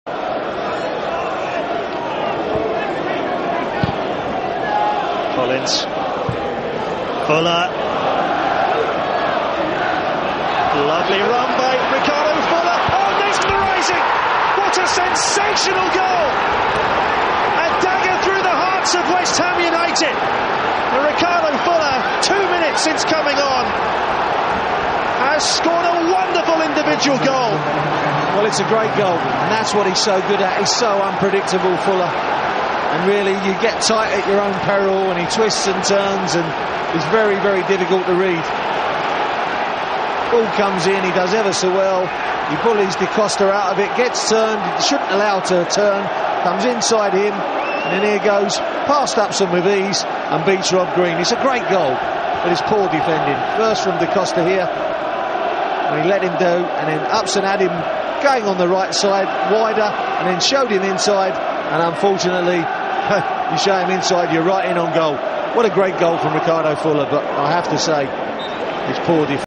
West ham VS Stoke city 0-1(Ricardo Fuller goal)
a great goal form fuller vs stoke city  this year 2009/2010
just like lionel messi :PpP